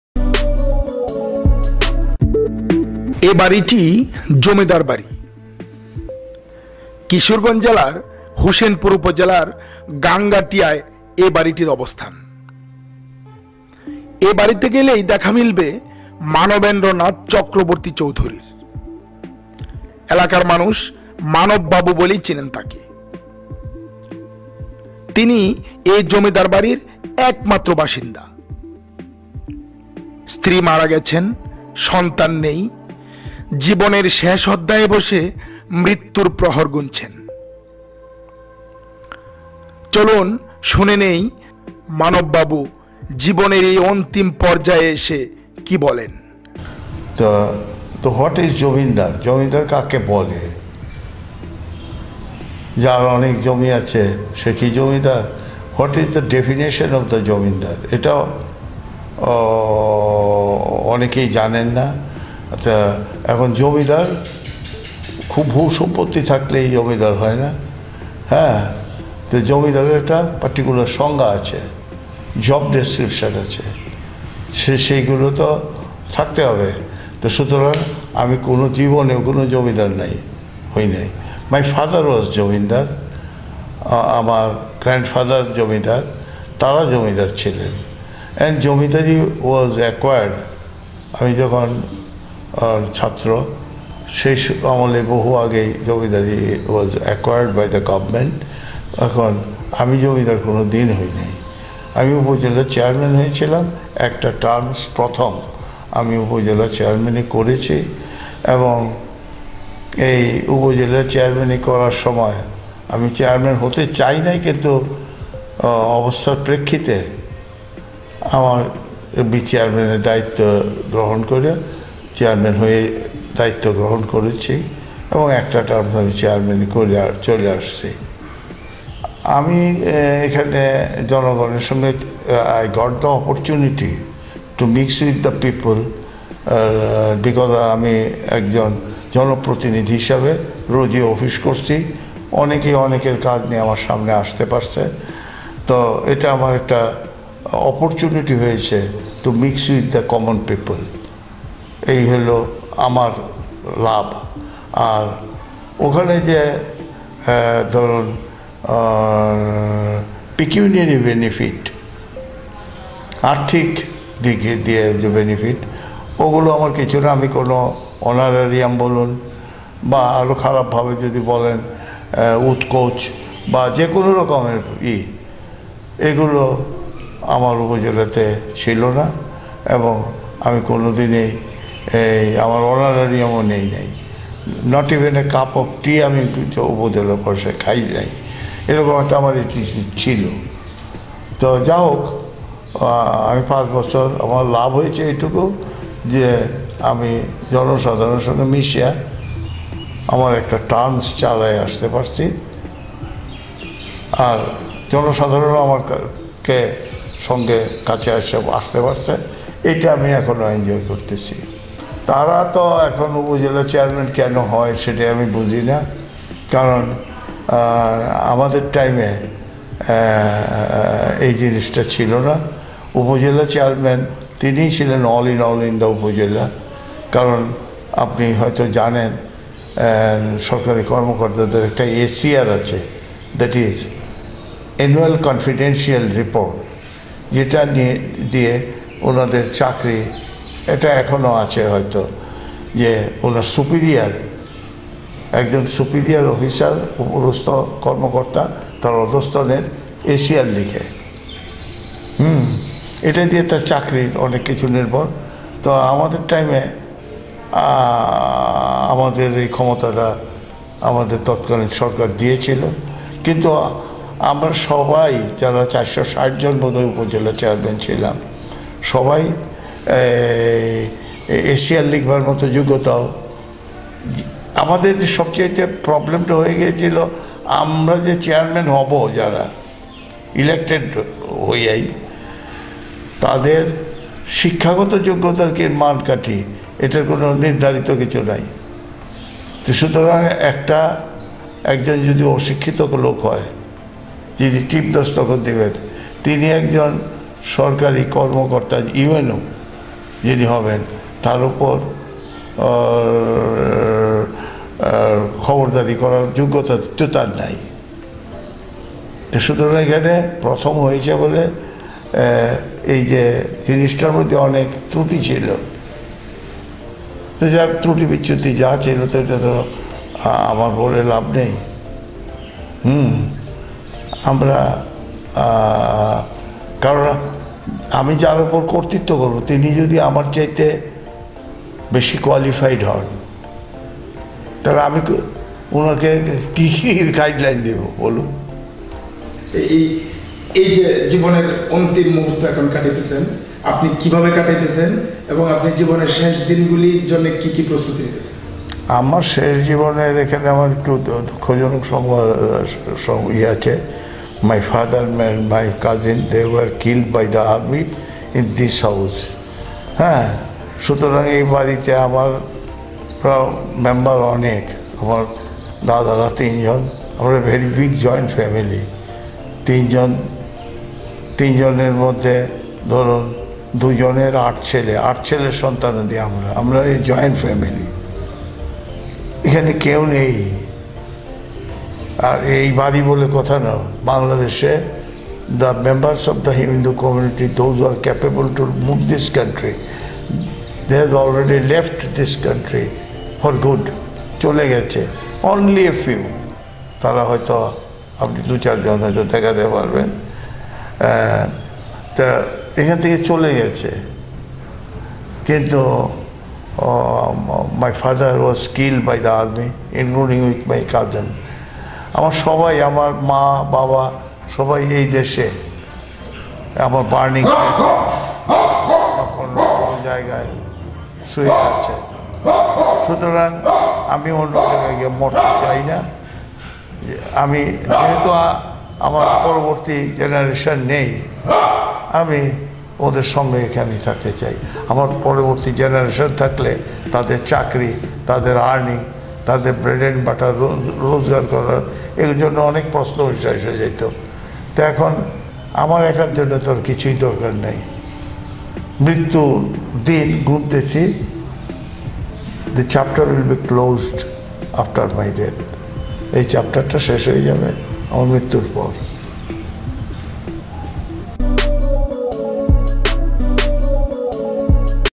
মানব বাবু | জীবিত জমিদার | Interview of the living zamindar | Manab Babu | spice24
জীবিত জমিদারের সাক্ষাৎকার

এটি জমিদার বাড়ি। কিশোরগঞ্জ জেলার হোসেনপুর উপজেলার গাঙ্গাটিয়ায় এ বাড়িটির অবস্থান।

এ বাড়িতে গেলেই দেখা মিলবে মানবেন্দ্র নাথ চক্রবর্তী চৌধুরীর। এলাকার মানুষ মানব বাবু বলেই চিনেন তাকে।

তিনি এ জমিদার বাড়ির একমাত্র বাসিন্দা।

স্ত্রী মারা গেছেন, সন্তান নেই। জীবনের শেষ অধ্যায়ে বসে মৃত্যুর প্রহর গুনছেন।

এ ভিডিওতে শুনুন, জমিদার পরিবারের একমাত্র জীবিত সদস্য মানব বাবু কি বলেন।